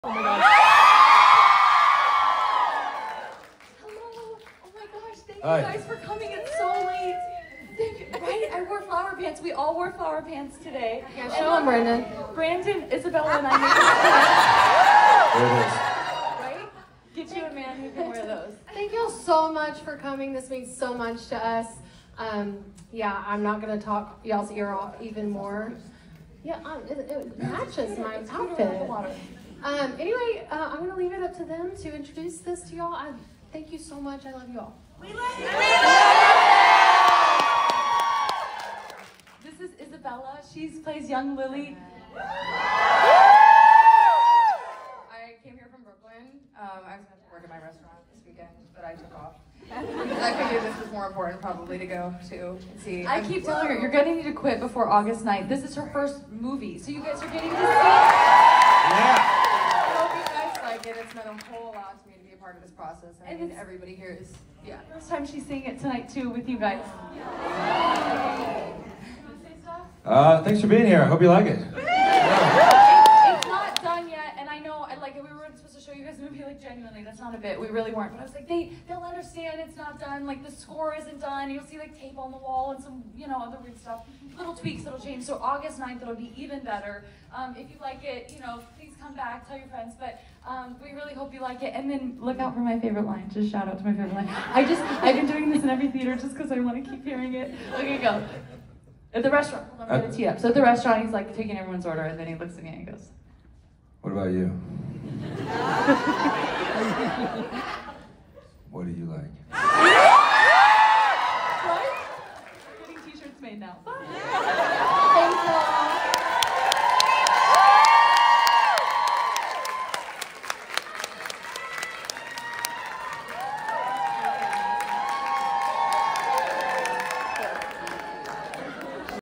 Early peek at It Ends With Us and Blake Lively!
Book Bonanza 2024!

I had a great week with everyone and a special surprise at the end! Watching It Ends With Us Early 🙌 Thank you Colleen Hoover!